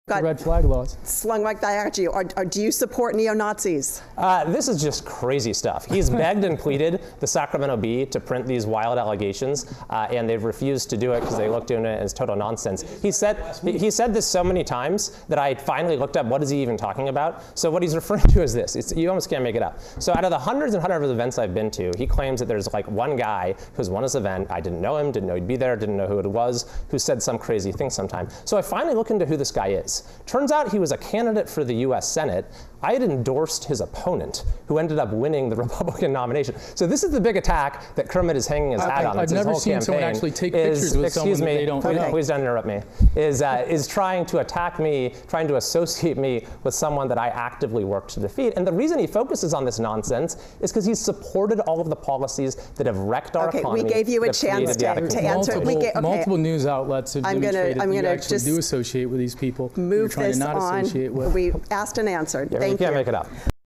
KCRA 3's Edie Lambert asks Kevin Kiley if he supports neo-Nazis
KCRA 3 and CapRadio hosted a live debate on Oct. 27, 2022, for Democrat Kermit Jones and Republican Kevin Kiley, the candidates for the 3rd Congressional District.

Jones is a doctor from Roseville and Kiley is currently a member of the State Assembly who represents parts of El Dorado, Placer and Sacramento counties.

The 3rd Congressional District does not have an incumbent after it was redrawn in California’s redistricting process. The district represents portions of Plumas, Mono, Sacramento Sierra, Yuba, Nevada, Placer, El Dorado, Alpine and Inyo counties.

Moderating the debate were KCRA 3 anchor Edie Lambert and CapRadio's Nicole Nixon.